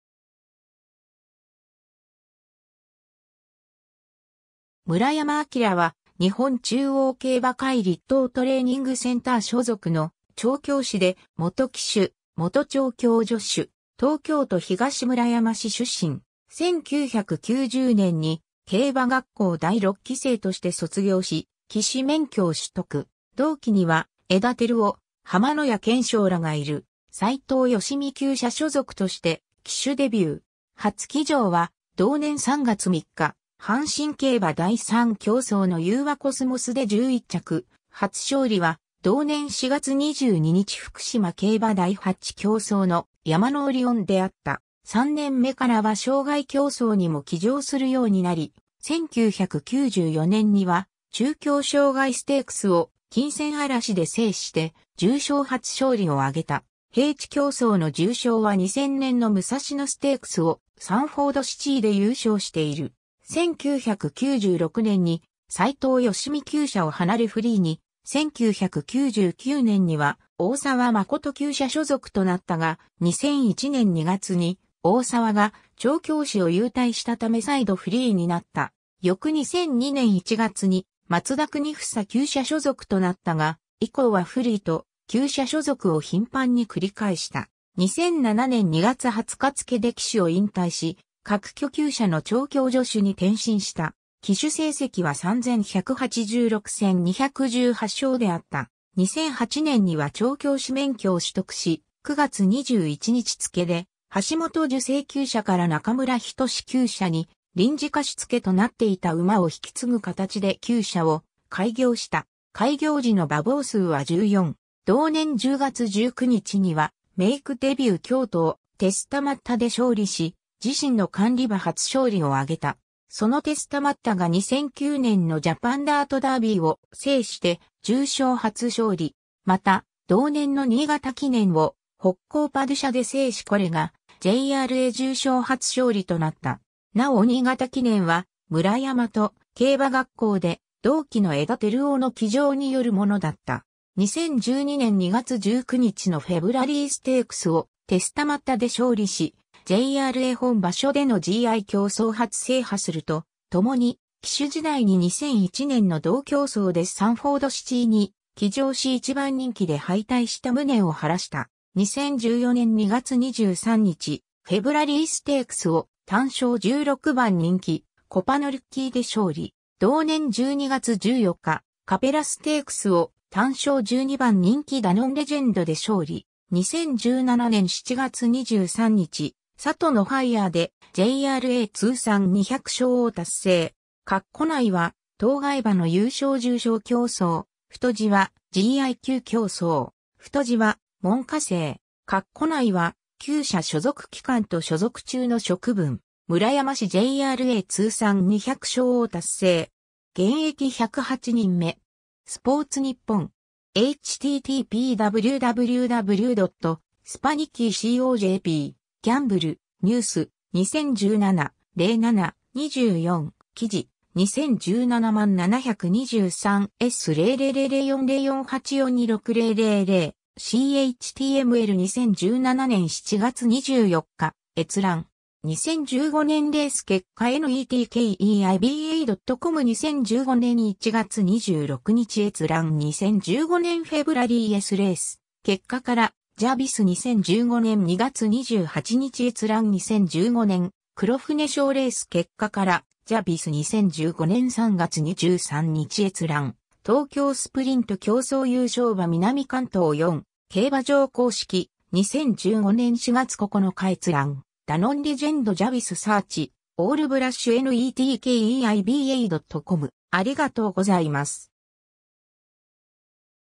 村山明 (競馬)
村山 明（むらやま あきら、1971年8月18日-）は日本中央競馬会 (JRA) 栗東トレーニングセンター所属の調教師で、元騎手、元調教助手。
東京都東村山市出身。
 1990年に競馬学校第6期生として卒業し、騎手免許を取得。
同期には江田照男、浜野谷憲尚らがいる。
斉藤義美厩舎所属として騎手デビュー。
初騎乗は同年3月3日阪神競馬第3競走のユーワコスモスで11着、初勝利は同年4月22日福島競馬第8競走のヤマノオリオンであった。
 3年目からは障害競走にも騎乗するようになり、1994年には中京障害ステークスをキンセンアラシで制して重賞初勝利を挙げた。
平地競走の重賞は2000年の武蔵野ステークスをサンフォードシチーで優勝している。
 1996年に斉藤義美厩舎を離れフリーに、1999年には大沢真厩舎所属となったが、2001年2月に大沢が調教師を勇退したため再度フリーになった。
翌2002年1月に松田国英厩舎所属となったが、以降はフリーと厩舎所属を頻繁に繰り返した。
 2007年2月20日付で騎手を引退し、角居厩舎の調教助手に転身した。
騎手成績は3186戦218勝（うち重賞2勝、障害戦222戦26勝）であった。
 2008年には調教師免許を取得し、9月21日付で橋本寿正厩舎から中村均厩舎に臨時貸付けとなっていた馬を引き継ぐかたちで厩舎を開業した。
開業時の馬房数は14。
 同年10月19日には、メイクデビュー京都をテスタマッタで勝利し、自身の管理馬初勝利をあげた。
そのテスタマッタが2009年のジャパンダートダービーを制して重賞初勝利。
また、同年の新潟記念をホッコーパドゥシャで制しこれがJRA重賞初勝利となった。
なお新潟記念は村山と競馬学校で同期の江田照男の騎乗（石橋脩の落馬負傷による乗り替わり）によるものだった。
 2012年2月19日のフェブラリーステークスをテスタマッタで勝利し、JRA本場所でのGI競走初制覇するとともに、騎手時代に2001年の同競走でサンフォードシチーに騎乗し1番人気で敗退した無念を晴らした。
 2014年2月23日、フェブラリーステークス（GI）を単勝16番人気コパノリッキー（田辺裕信騎手騎乗）で勝利。
 同年12月14日、カペラステークス（GIII）を単勝12番人気ダノンレジェンド（丸田恭介騎手騎乗）で勝利。
 2017年7月23日、サトノファイヤーでJRA通算200勝を達成。
 ※括弧内は当該馬の優勝重賞競走、太字はGI級競走。
 ※太字は門下生。
括弧内は厩舎所属期間と所属中の職分。
^ “村山師　ＪＲＡ通算２００勝を達成！現役１０８人目”. スポーツニッポン (Sponichi Annex). (2017年7月24日). 2017年7月24日閲覧。
 ^ 2015年レース結果netkeiba.com　2015年1月26日閲覧 ^ 2015年フェブラリーSレース結果 - JBIS　2015年2月28日閲覧 ^ 2015年黒船賞レース結果 - JBIS　2015年3月23日閲覧 ^ 東京スプリント競走優勝馬 - 南関東4競馬場公式、2015年4月9日閲覧 ^ ダノンレジェンドJBISサーチ ^ オールブラッシュnetkeiba.com